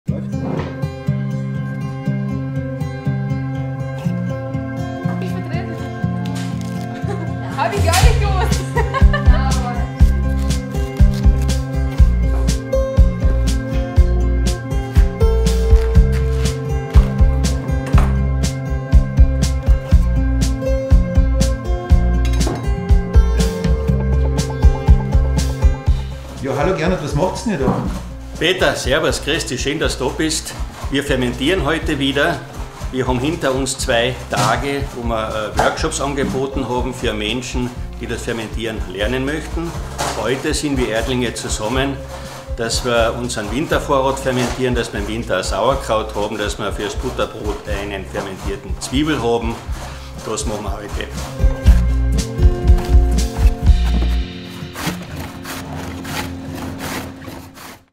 Fermentieren in der Erde
🌽 Gemüse fermentieren in der Erde 😍

🤔 Hier ein paar Gründe, warum auch du das Fermentieren einmal ausprobieren solltest:

🥕 Wenn du dein fermentiertes Gemüse roh verzehrst, dann nimmst du damit auch Millionen von lebenden Milchsäurebakterien zu dir. Diese können deine Gesundheit, deine Darmflora und dein Immunsystem positiv beeinflussen. 😌

🥫 Das Gemüse ist länger haltbar. So kannst du Sommergemüse beispielsweise auch in den Wintermonaten essen. Lecker und praktisch zugleich. 🍅

🤤 Es entstehen neue, einzigartige Aromen. Jedes Ferment schmeckt durch die natürliche Gärung immer etwas verschieden. Genau das macht es so geschmacklich individuell und besonders. 💚

🥗 Vitamine bleiben im Rohzustand bestehen. Das Gemüse wird beim Fermentieren nicht erhitzt, wodurch alle Vitamine enthalten bleiben. 🌶

🔌 Ja und in Zeiten wie diesen muss unbedingt erwähnt werden, dass das Fermentieren eine Methode ist, mit der man ohne Energie-Einsatz Lebensmittel lange haltbar machen kann. ☝️